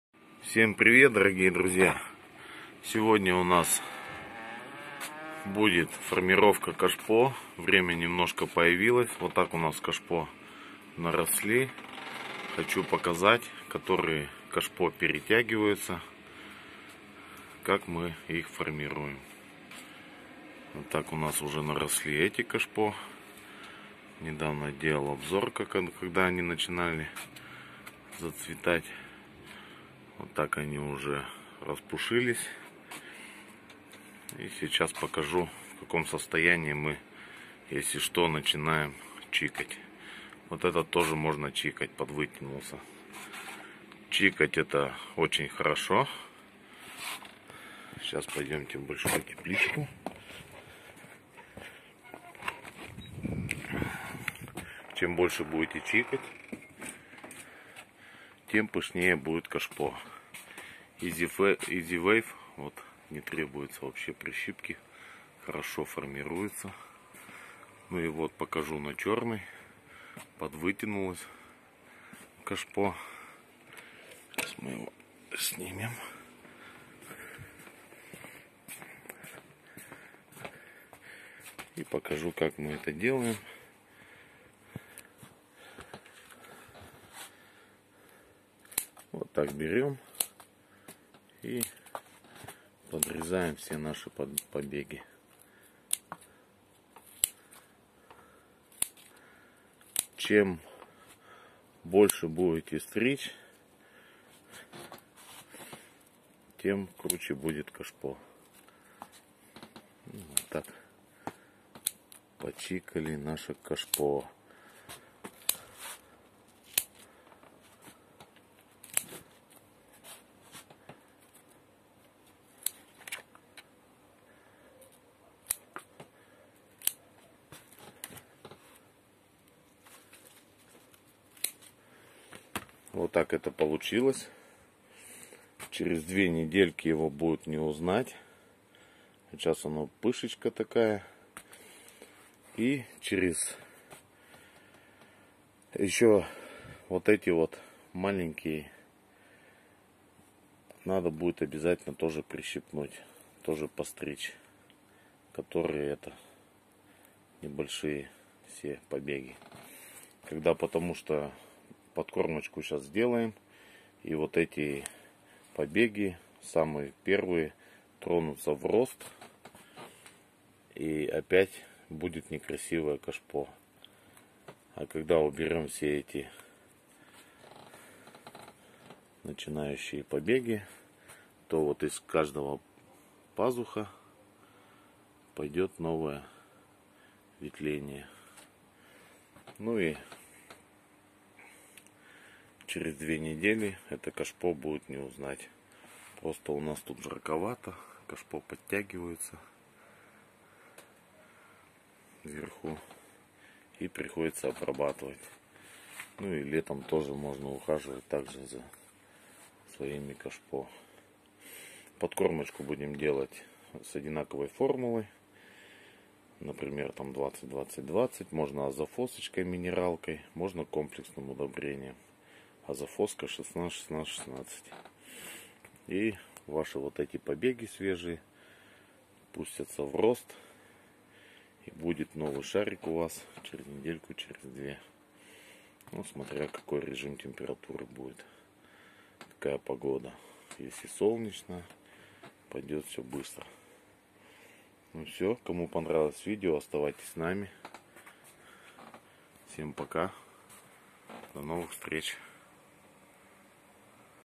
КАК МЫ ФОРМИРУЕМ ПЕТУНИИ В КАШПО? ЭТО НАДО ВИДЕТЬ!
ВСЕМ ПРИВЕТ ДОРОГИЕ ДРУЗЬЯ!!! Хотел бы показать как мы формируем свои петунии в кошпо.